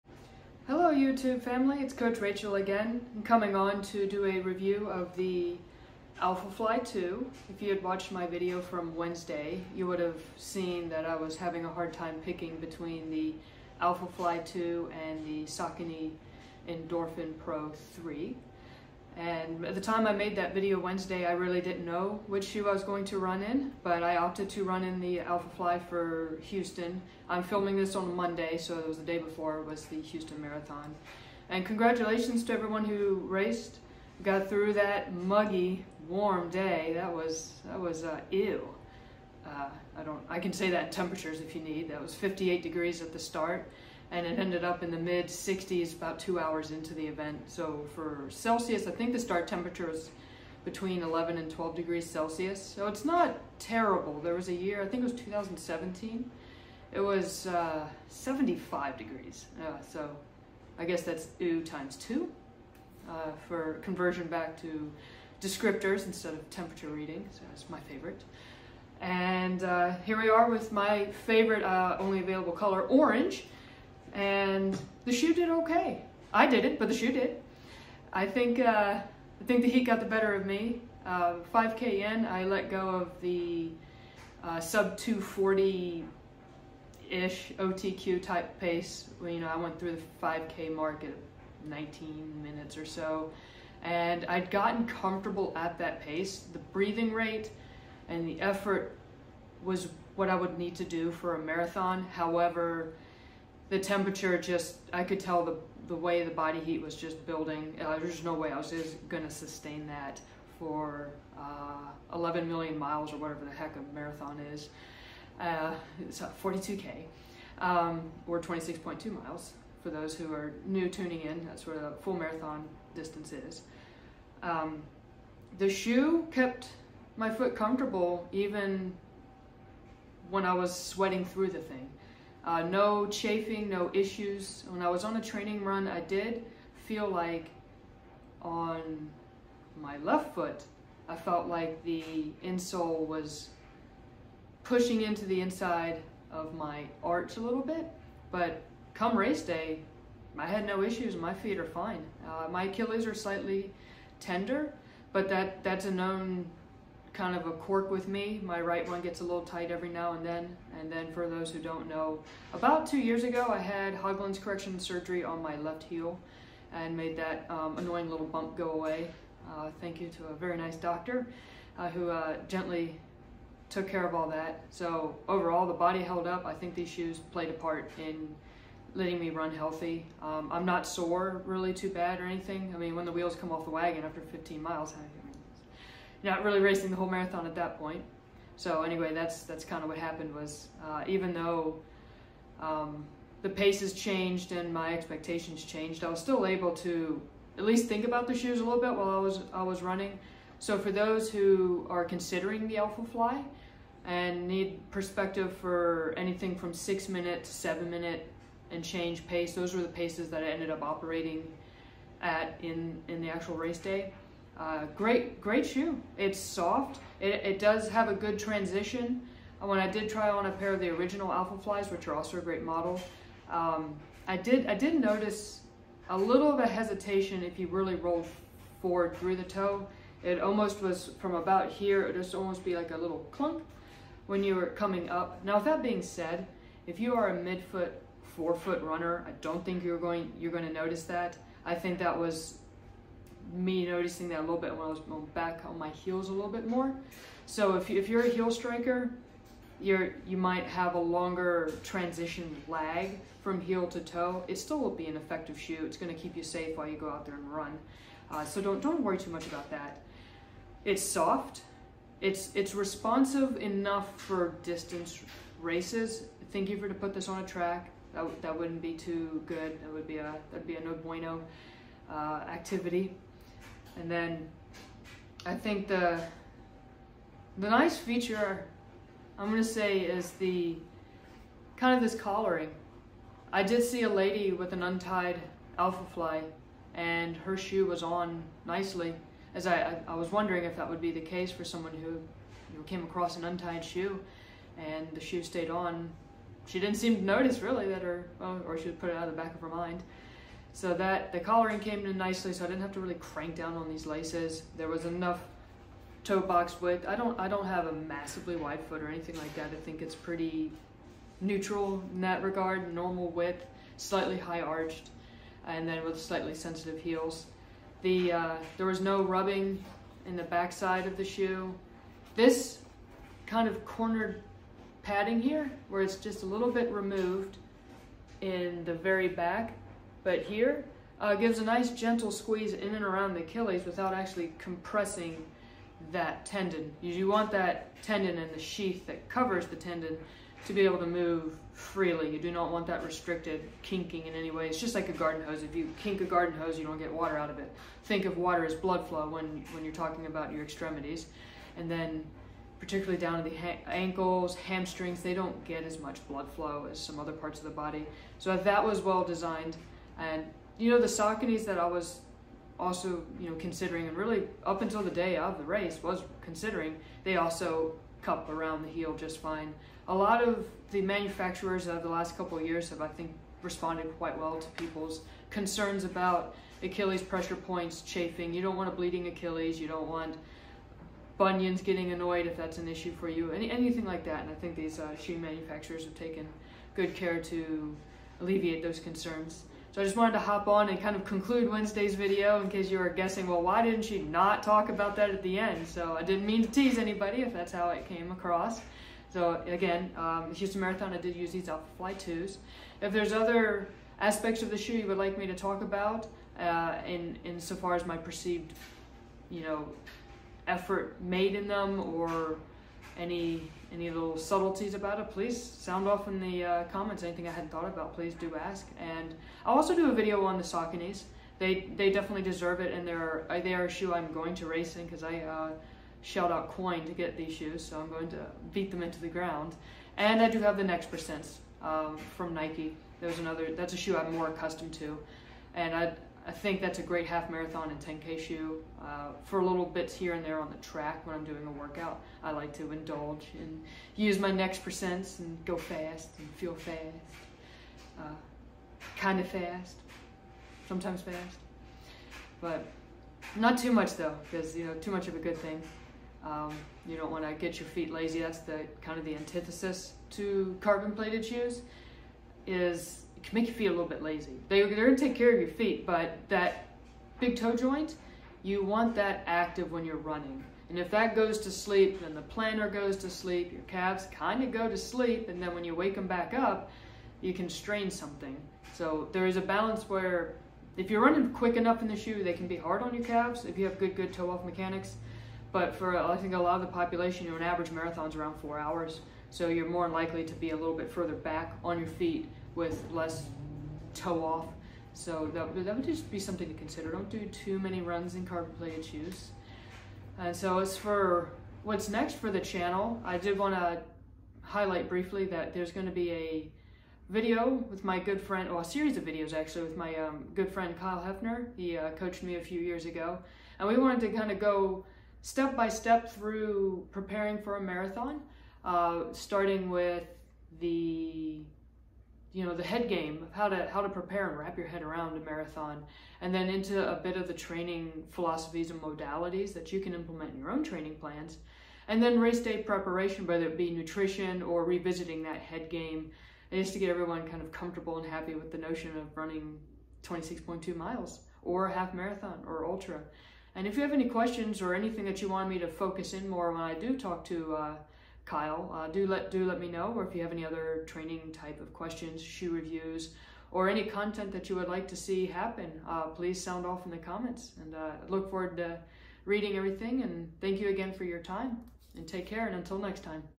Nike Alphafly Next% 2 Marathon Race Review
Hi everyone. In this second upload, I review the Nike Alphafly II and break down my Houston Marathon Race. This was a solid shoe on a challenging and warm day!
Please let me know if you have any questions or input for some training topics for future videos.